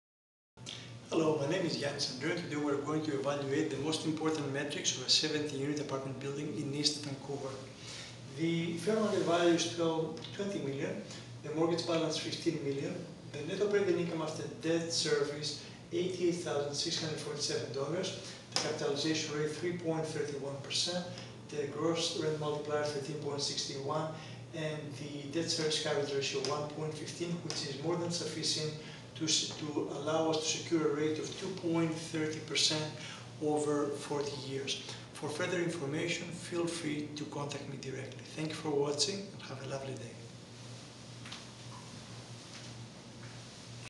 East Vancouver 70-Unit Apartment Building Best Mortgage Solution with an A Lender - Rate of 2.30%
The video focuses on the most important Metrics used to evaluate the purchase of an Apartment Building.  The key metrics the video focuses on are the following: 1) FMV(Fair Market Value) or Sale Price, 2) Number of Units, 3) Gross Revenue, 4) Total Expenses, 5) NOI(Net Operating Income, 6) Down payment , 7) Annual Mortgage Payments, 8) NOI after debt service, 9) Capitalization Rate(NOI / Market Value), 10) Gross Rent Multiplier(Market Value / Gross Rent), 11) DSCR = Debt Service Coverage Ratio(NOI / Annual Debt Service). It is highly recommended that you verify all income and expense categories on your own before you finalize the purchase of any Apartment Building.